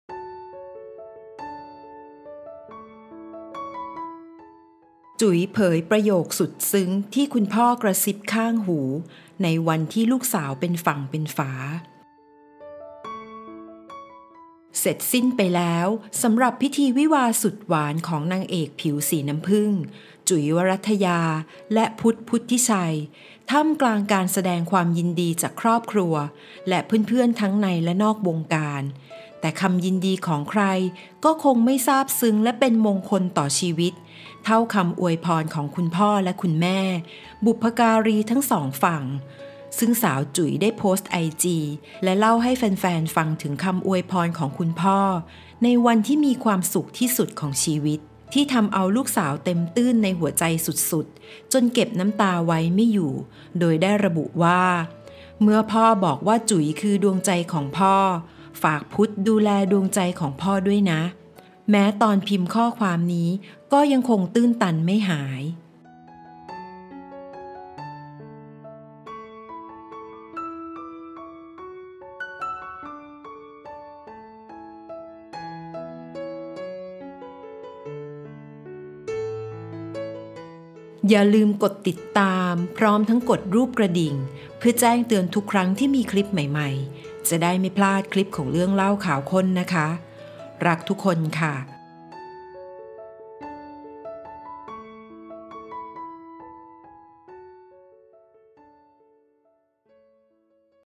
ประโยคสุดซึ้ง! ที่คุณพ่อ จุ๋ย กระซิบข้างหู ในวันที่ลูกสาว เป็นฝั่งเป็นฝา
เสร็จสิ้นไปแล้วสำหรับพิธีวิวาห์สุดหวานของ นางเอกผิวสีน้ำผึ้ง จุ๋ย วรัทยา และ พุฒ พุฒิชัย ท่ามกลางการแสดงความยินดีจากครอบครัวและเพื่อนๆทั้งในและนอกวงการ  แต่คำยินดีของใครก็คงไม่ซาบซึ้งและเป็นมงคลต่อชีวิตเท่าคำอวยพรของคุณพ่อ-คุณแม่ บุพการีทั้งสองฝั่ง

ซึ่งสาวจุ๋ย ได้โพสต์IG และเล่าให้แฟนๆฟังถึงคำอวยพรของคุณพ่อในวันที่มีความสุขที่สุดของชีวิต ที่ทำเอาลูกสาวเต็มตื้นในหัวใจสุดๆจนเก็บน้ำตาไว้ไม่อยู่ว่า